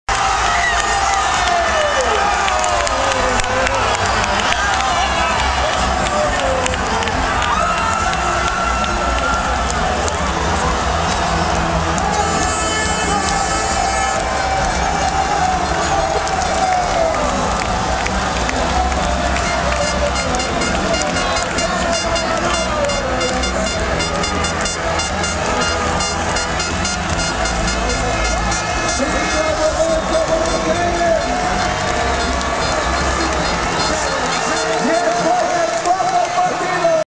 Celebrating Fabregas's goal againt Italy at the fan zone of Madrid
Match Italy v Spain, group stage